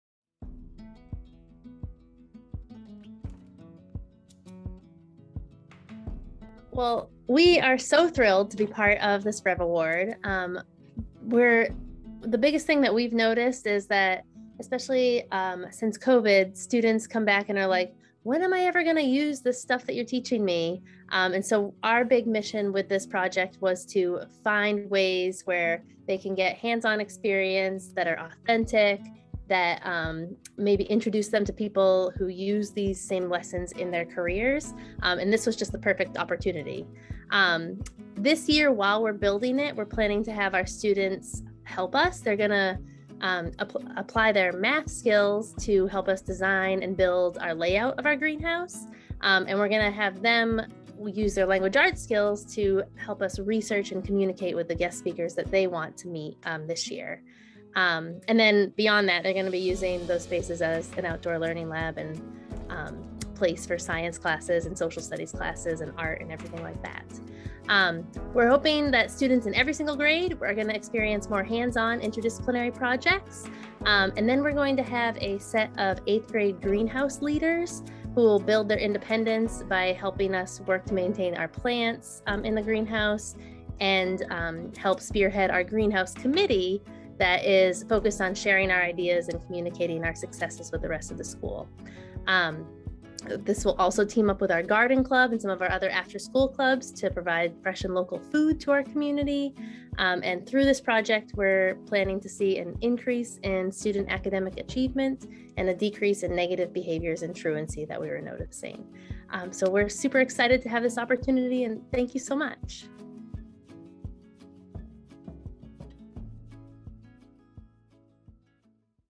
RREV Project: Lake Region Middle School Outdoor Education Spaces
The Maine Department of Education (DOE) awarded $1.6 million in Rethinking Responsive Education Ventures (RREV) funding to support education innovation in twelve school administrative units (SAUs) across Maine. These federal funds will be used to invest in strategies to engage students through outdoor learning, alternative education, online learning pathways, and more.

Awardees for this fourth round of RREV funding include RSU 35 Marshwood Great Works School, RSU 34 Old Town Middle/High School, RSU 73 Spruce Mountain Elementary School, Gorham High School, MSAD 61 Lake Region Middle School, MSAD 11 Gardiner High School, Limestone Community School, Maine Academy of Natural Sciences, Wayfinder Schools, RSU 13 Oceanside High School, MSAD 49 Lawrence High School, and MSAD 59 Madison Elementary School. The first round of RREV investments were made last fall, a second round in March, a third round in June, and total RREV investments now near $8 million with 39 awardees.

Lake Region Middle School will build both a large greenhouse and an outdoor learning pavilion to increase their capacity for outdoor education and ensure that these spaces are ADA accessible.